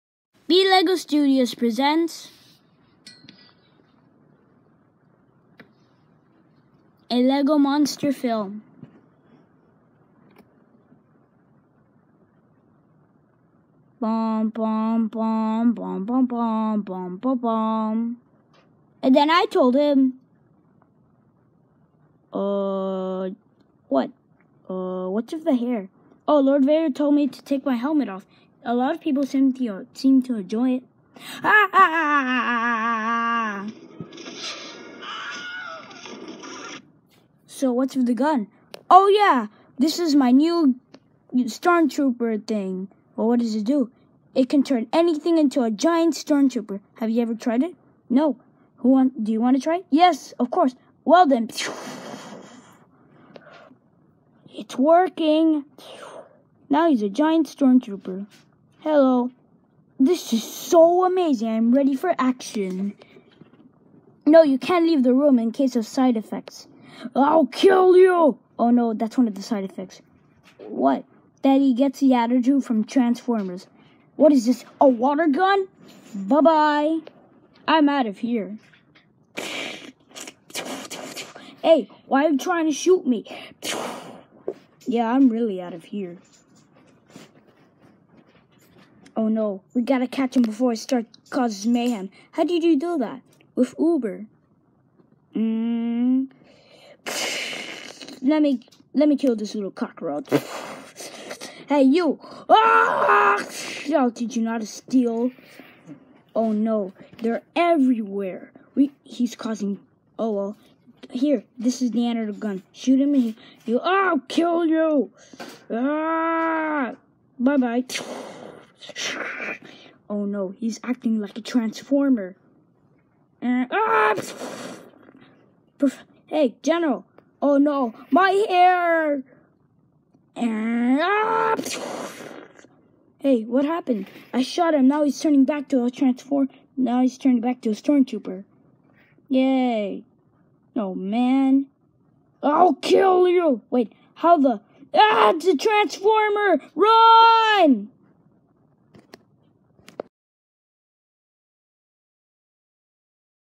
The Giant Stormtrooper
After getting hit by a ray gun he turns into a giant Stormtrooper but he starts acting like a Transformer  and it's up to a Battle  Droid and a general to stop him